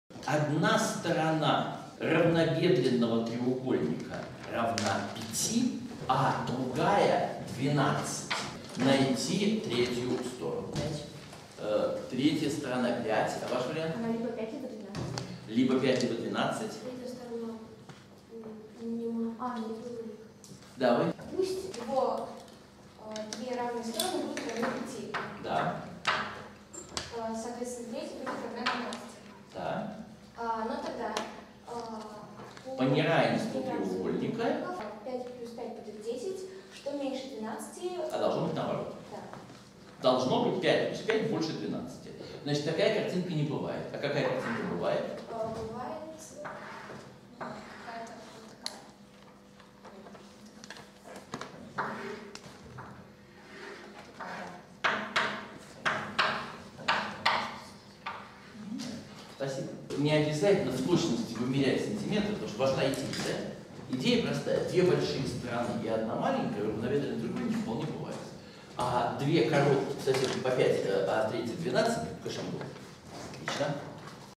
2 Равнобедренный треугольник со сторонами 5 и 12
Длина одной из сторон равнобедренного треугольника равна 5, а длина другой равна 12.  Какова длина третьей стороны?